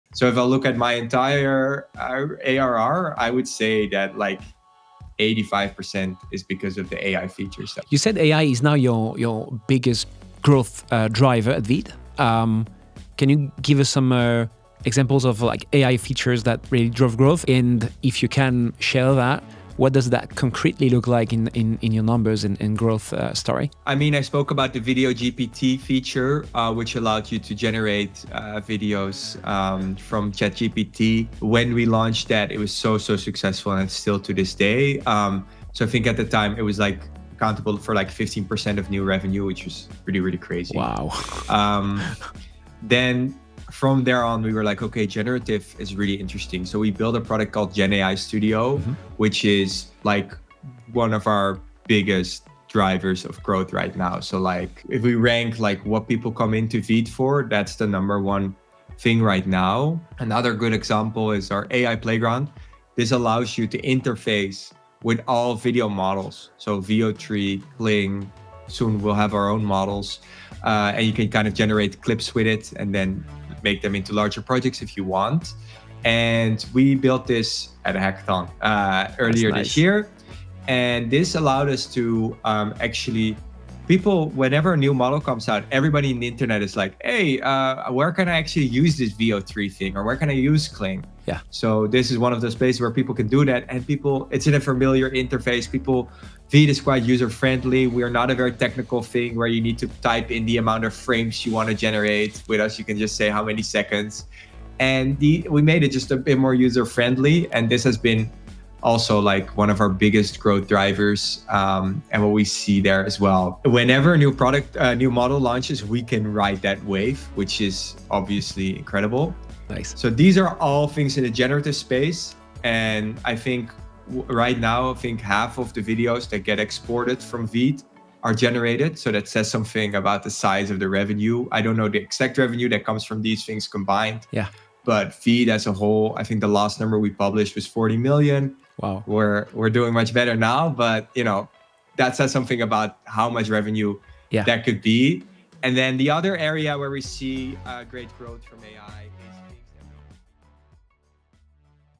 85% of ARR with AI Features 🤯
Full episode on my channel.

In this episode, I sit down with Samuel Beek, CPO at Veed.

Together we explore how they went from a video editing tool back in 2018, to an AI First company making 85% of ARR on AI features.